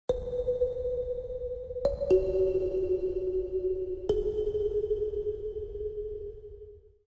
FOLD: PUCK WALL ART: A Wall Arts collection by VIBIA
FOLD is a quadruple mounted, geometrically shaped wall light, designed by Arik Levy